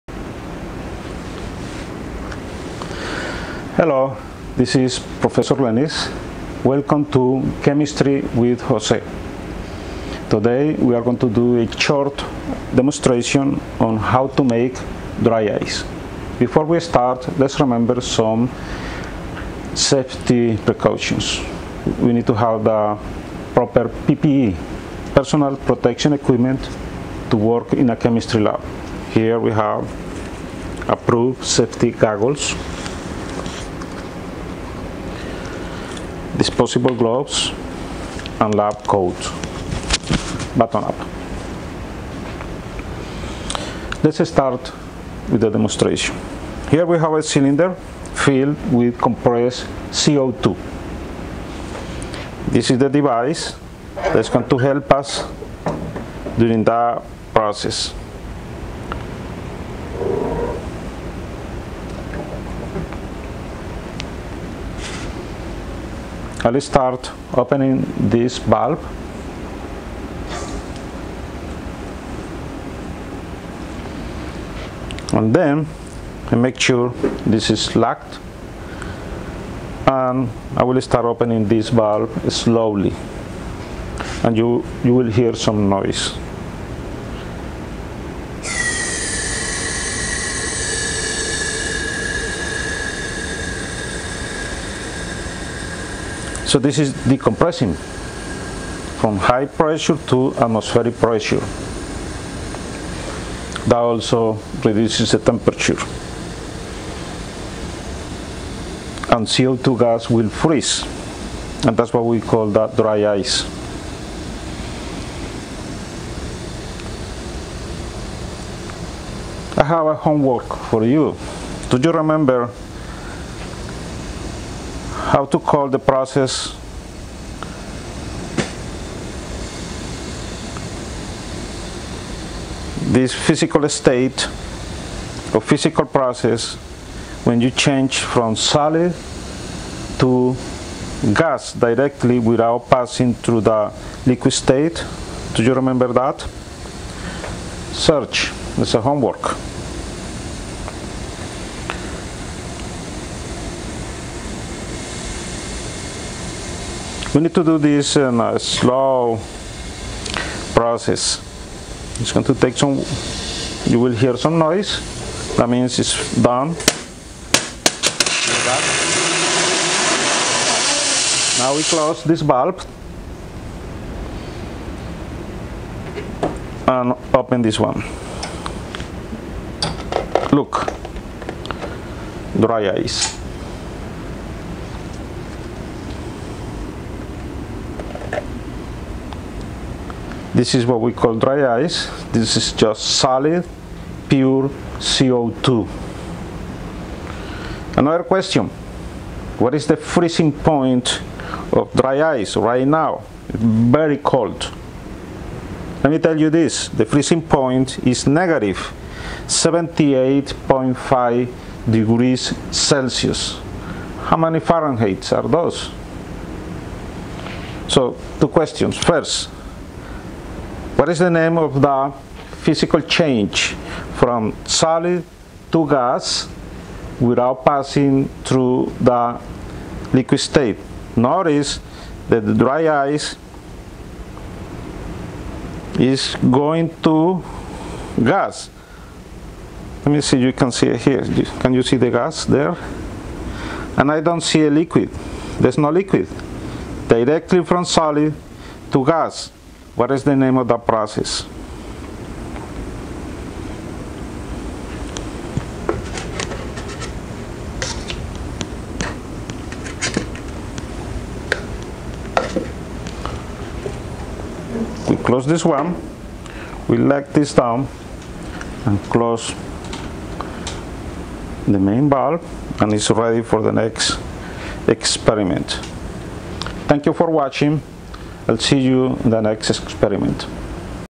Making Dry Ice - Solid CO2 in the Lab
This video shows how to make solid carbon dioxide, commonly known as "dry ice", in the lab from compressed CO2(liq). the liquefied CO2 goes through an expansion valve to a chamber at atmospheric pressure where the liquid CO2 changes quickly to gas. This quick state change is accompanied with a dramatic drop of temperature and about 45%  of the gas freezes at -78.5 C (-109.3F).
Sublimation is a physical change where the solid state changes directly into a gas state without going through the liquid state.